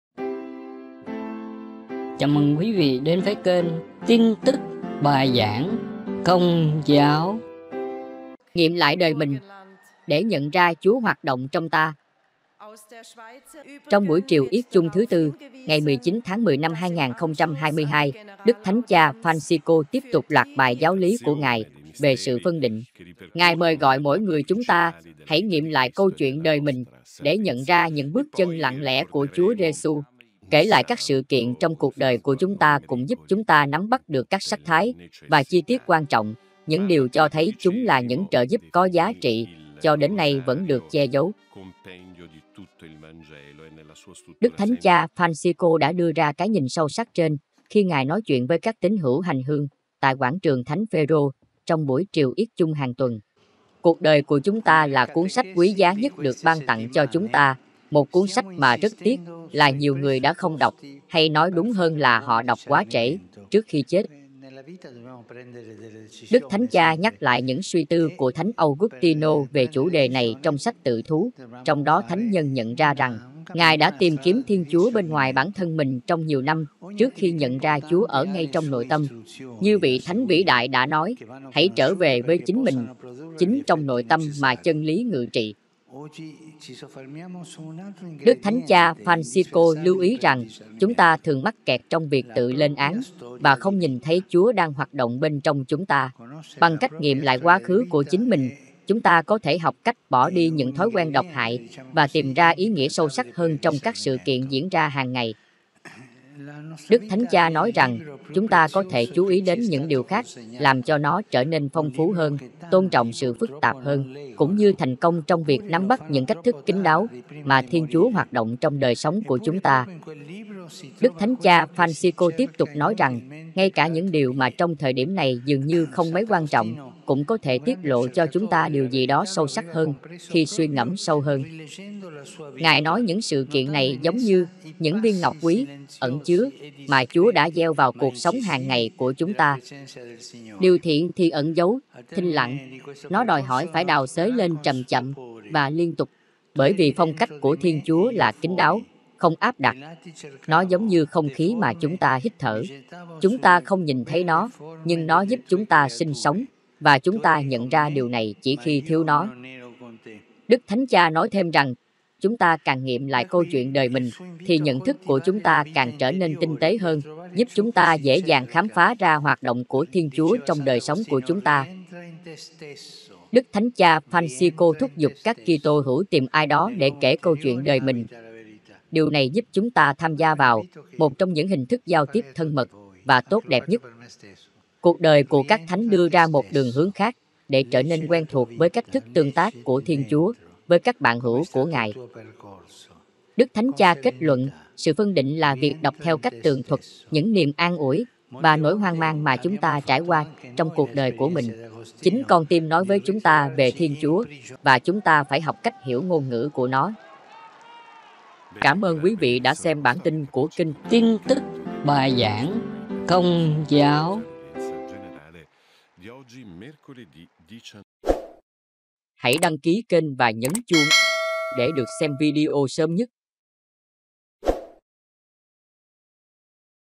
Nghiệm Lại Đời Mình Để Nhận Ra CHÚA Hoạt Động Trong Ta - Tin Tức Công Giáo
✝️ Nghiệm Lại Đời Mình Để Nhận Ra CHÚA Hoạt Động Trong Ta

✍️ Trong buổi triều yết chung thứ Tư 19/10/2022, Đức Thánh Cha Phanxicô tiếp tục loạt bài giáo lý của ngài về sự phân định. Ngài mời gọi mỗi người chúng ta hãy nghiệm lại câu chuyện đời mình để khám phá ra những bước chân lặng lẽ của Chúa Giêsu.

 ✍️ “Kể lại các sự kiện trong cuộc đời của chúng ta cũng giúp chúng ta nắm bắt được các sắc thái và chi tiết quan trọng, những điều có thể tiết lộ chúng là những trợ giúp có giá trị, cho đến nay vẫn được che giấu.”

✍️ Đức Thánh Cha Phanxicô đã đưa ra cái nhìn sâu sắc trên khi ngài nói chuyện với các tín hữu hành hương tại Quảng trường Thánh Phêrô trong buổi triều yết chung hàng tuần.

✍️ “Cuộc đời của chúng ta là ‘cuốn sách’ quý giá nhất được ban tặng cho chúng ta, một cuốn sách mà rất tiếc là nhiều người đã không đọc, hay nói đúng hơn là họ đọc quá trễ, trước khi chết”.

📖 Chân lý ngự trị trong nội tâm

✍️ Đức Thánh Cha nhắc lại những suy tư của Thánh Augustinô về chủ đề này trong Sách Tự Thú, trong đó thánh nhân nhận ra rằng ngài đã tìm kiếm Thiên Chúa bên ngoài bản thân mình trong nhiều năm trước khi khám phá ra Chúa ở ngay trong nội tâm.

✍️ Như vị Thánh vĩ đại đã nói, “Hãy trở về với chính mình. Chính trong nội tâm mà Chân lý ngự trị”.

✍️ Đức Thánh Cha Phanxicô lưu ý rằng chúng ta thường mắc kẹt trong việc tự lên án và không nhìn thấy Chúa đang hoạt động bên trong chúng ta.

✍️ Bằng cách nghiệm lại quá khứ của chính mình, chúng ta có thể học cách bỏ đi những thói quen “độc hại” và tìm ra ý nghĩa sâu sắc hơn trong các sự kiện diễn ra hàng ngày.

✍️ Đức Thánh Cha nói rằng chúng ta có thể “chú ý đến những điều khác, làm cho nó trở nên phong phú hơn, tôn trọng sự phức tạp hơn, cũng như thành công trong việc nắm bắt những cách thức kín đáo mà Thiên Chúa hoạt động trong đời sống của chúng ta.”

📖 Khám phá sự thinh lặng tiềm ẩn của điều thiện

✍️ Đức Thánh Cha Phanxicô tiếp tục nói rằng ngay cả những điều mà trong thời điểm này dường như không mấy quan trọng cũng có thể tiết lộ cho chúng ta điều gì đó sâu sắc hơn khi suy ngẫm sâu hơn.

✍️ Ngài nói rằng những sự kiện này giống như “những viên ngọc quý, ẩn chứa mà Chúa đã gieo” vào cuộc sống hàng ngày của chúng ta.

✍️ “Điều thiện thì ẩn giấu, thinh lặng. Nó đòi hỏi phải đào xới lên chầm chậm và liên tục. Bởi vì phong cách của Thiên Chúa là kín đáo, không áp đặt; nó giống như không khí mà chúng ta hít thở. Chúng ta không nhìn thấy nó, nhưng nó giúp chúng ta sinh sống, và chúng ta nhận ra điều này chỉ khi thiếu nó”.

✍️ Đức Thánh Cha nói thêm rằng chúng ta càng nghiệm lại câu chuyện đời mình, thì nhận thức của chúng ta càng trở nên tinh tế hơn, giúp chúng ta dễ dàng khám phá ra hoạt động của Thiên Chúa trong đời sống của chúng ta.

📖 Những cuộc trò chuyện thân mật

✍️ Đức Thánh Cha Phanxicô thúc giục các Kitô hữu tìm ai đó để kể câu chuyện đời mình, điều này giúp chúng ta tham gia vào “một trong những hình thức giao tiếp thân mật và tốt đẹp nhất”.

✍️ Cuộc đời của các Thánh đưa ra một đường hướng khác để trở nên quen thuộc với cách thức tương tác của Thiên Chúa với các bạn hữu của Ngài.

✍️ Đức Thánh Cha kết luận: “Sự phân định là việc đọc theo cách tường thuật những niềm an ủi và nỗi hoang mang mà chúng ta trải qua trong cuộc đời của mình. Chính con tim nói với chúng ta về Thiên Chúa, và chúng ta phải học cách hiểu ngôn ngữ của nó.”

➡️ Nguồn: Vatican News
✍️ Tạ Ân Phúc